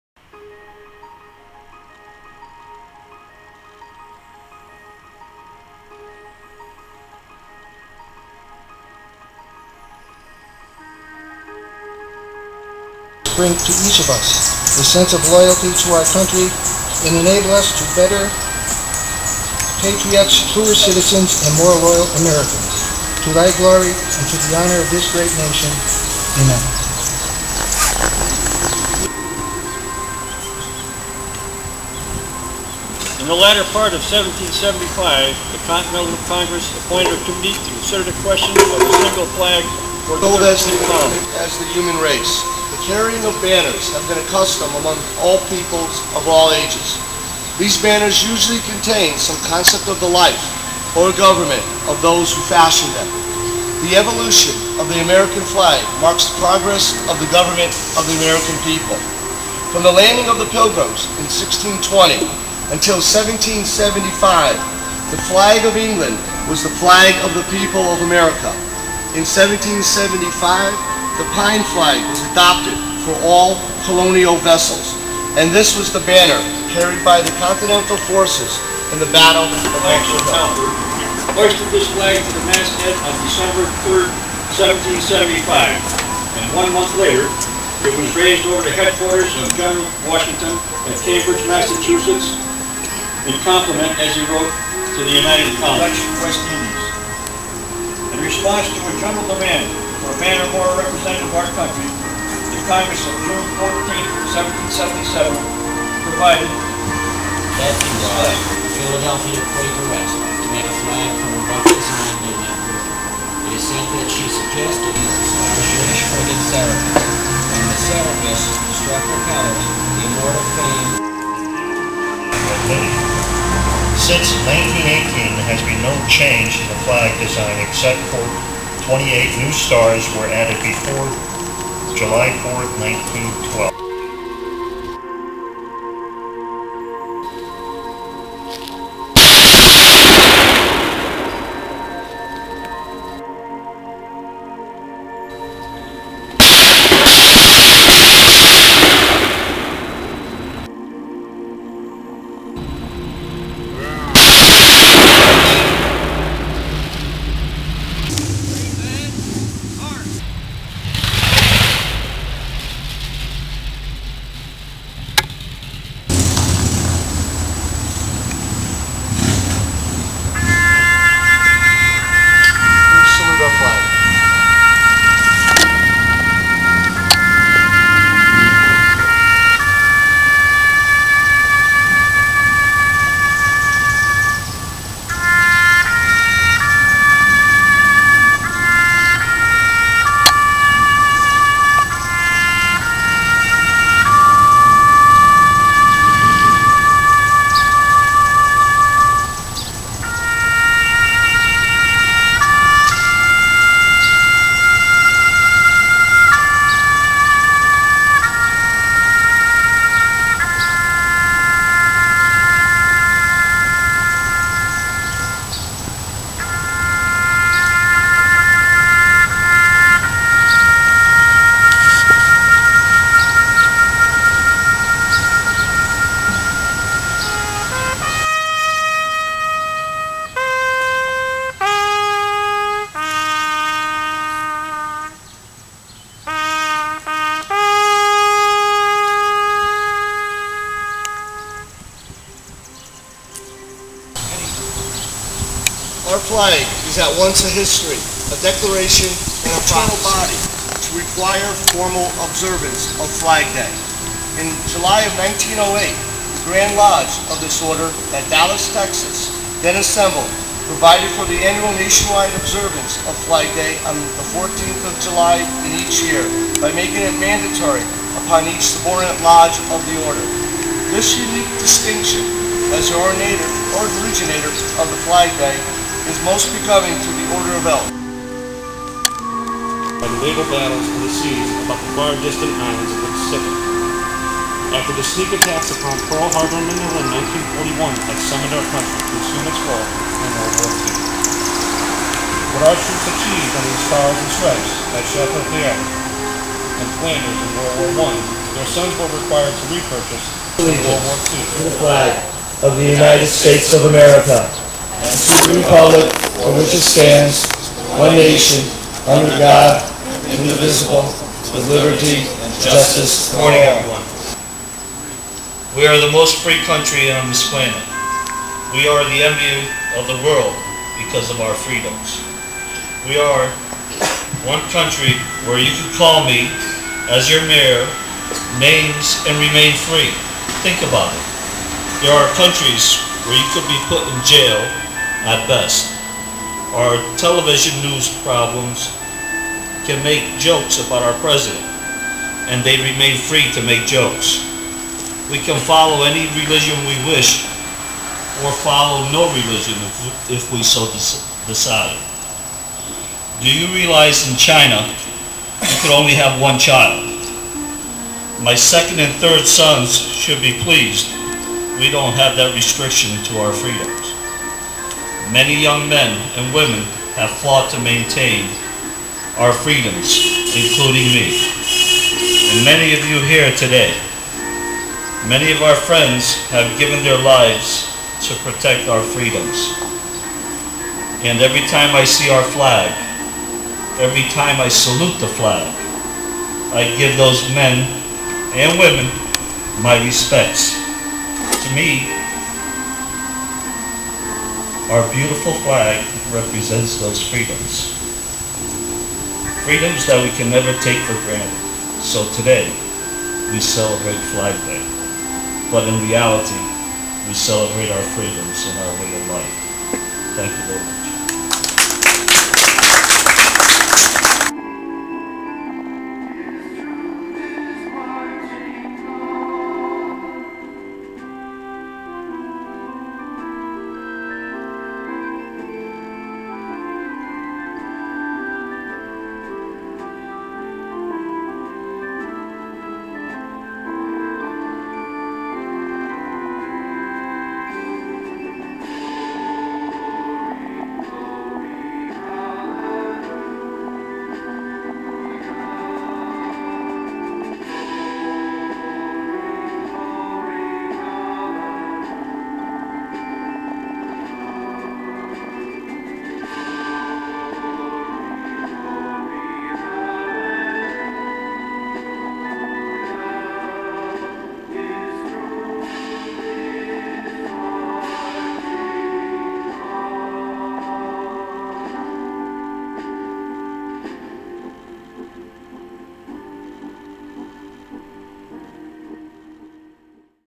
Flag Day 2007 in Derby, Connecticut
Video of the Flag Day Ceremony on the Derby Green hosted by the Derby Lodge of Elks Lodge #571.